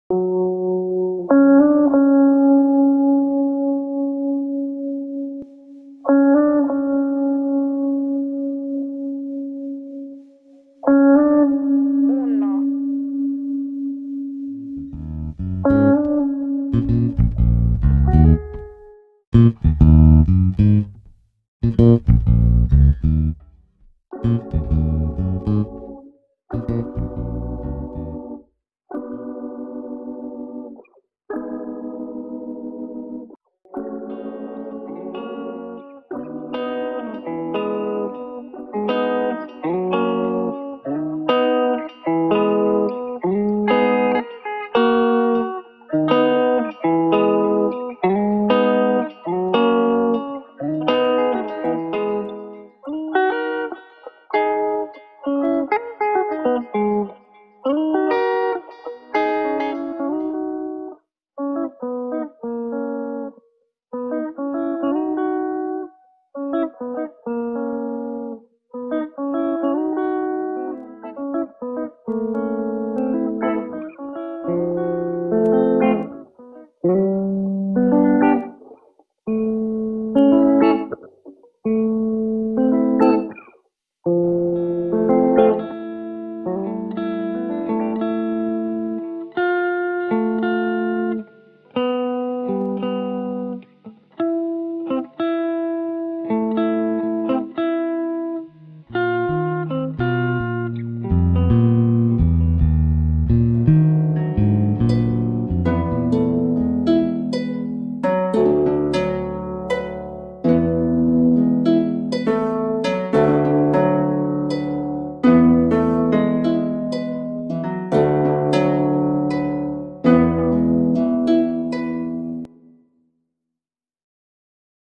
Free Guitar Loops (2022)
In 2020 I put out free Afrohighlife guitar loops and ever since I have out that loop, I have been getting DMS requesting I put out more.

well here you have it.

40 free Guitar sample loops!!!
Played and recorded by- unofini